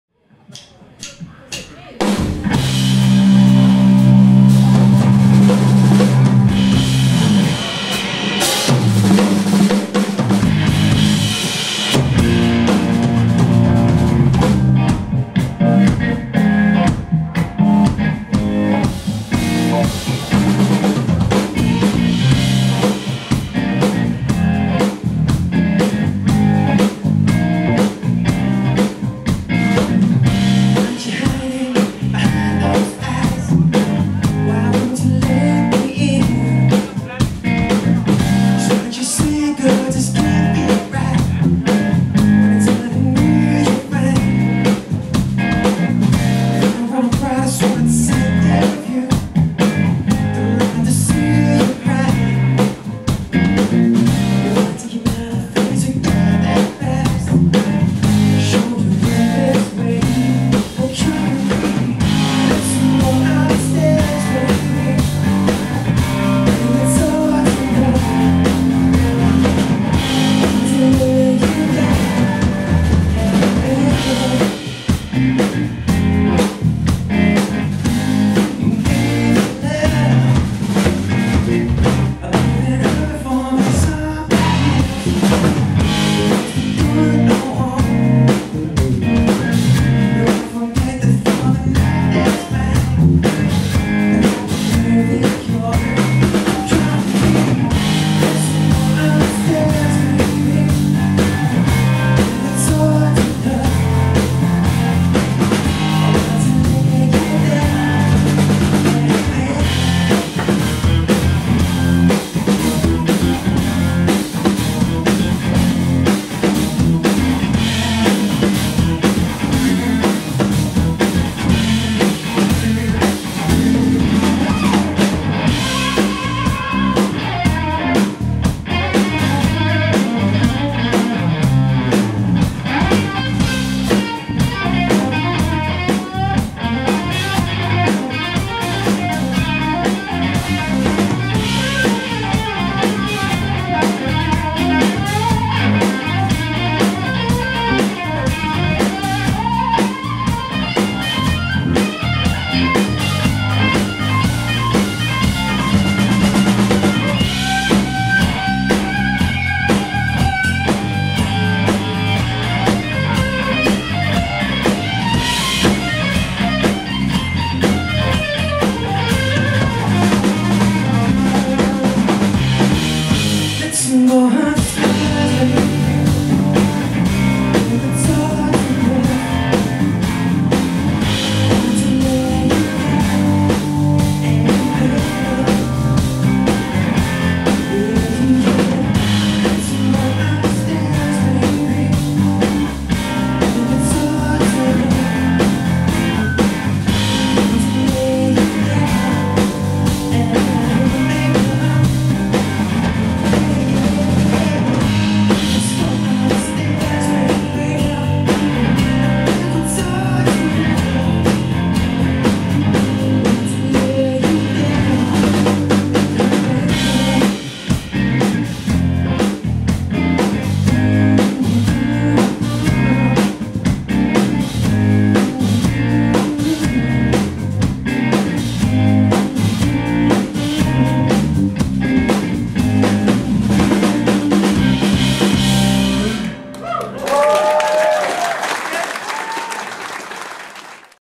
Ben Poole @ Downtown Bluesclub Hamburg 2019 (unseen)
A reminder what a great night we all had at this venue back on the 16/11/2019